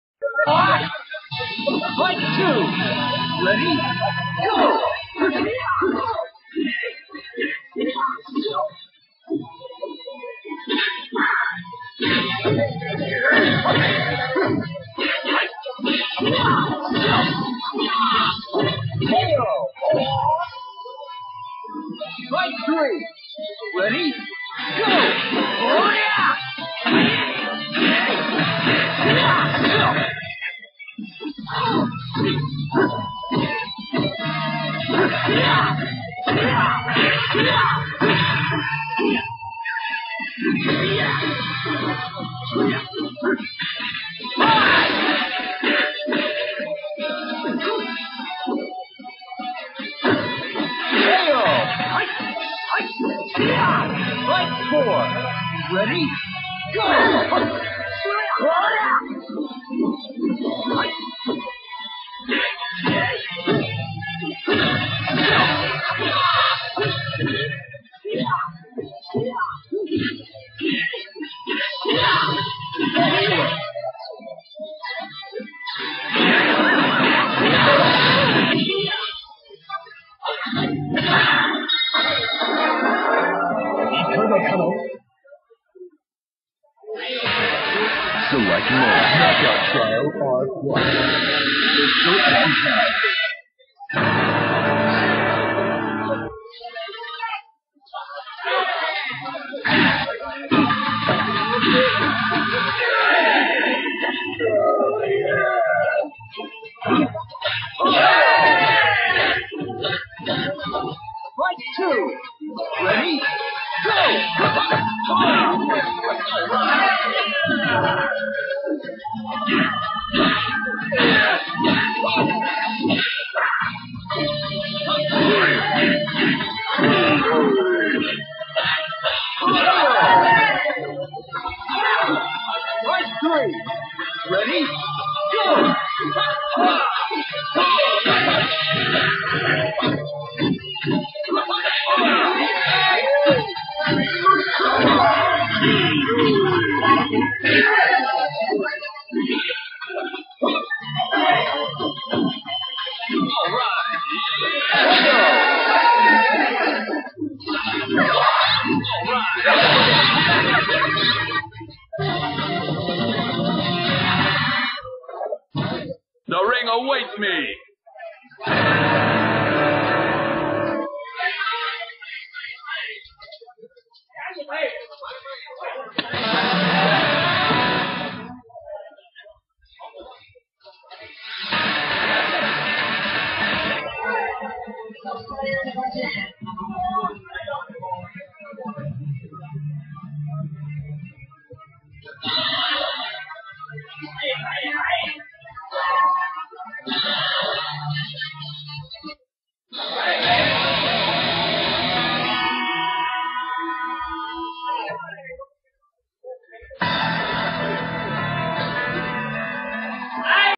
自作マニア＠にぶいち(AK) x ゾンレノン(LE)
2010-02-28ゲームニュートン大山店（03−3554−2668）にて行われたTHE NEWTON CUPのプレイオフ対戦動画です。